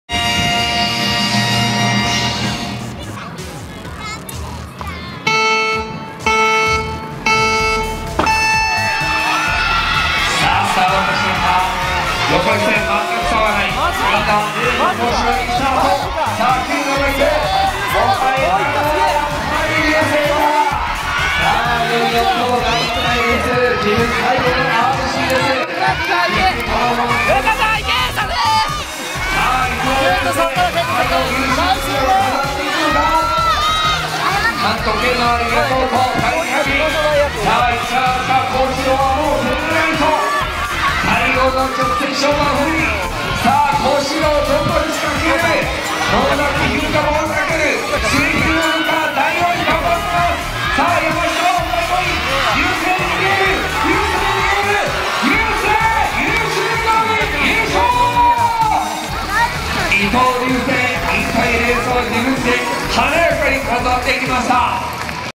日本STRIDER RACING富士站 六歲男童決勝賽，熱血激戰！ BIKEfun拜訪單車
「六歲的時候，你在做什麼？」
看卡通？ 跟鄰居在公園玩溜滑梯？
全世界最認真的日本人裡...
有群孩子的童年很不一樣。

日本STRIDER RACING富士站
六歲男童決勝賽，熱血激戰！

STRIDER滑步車，今日各色到貨！！

影片來源：第6戦 富士 2017 5 7 〜 6歳A決勝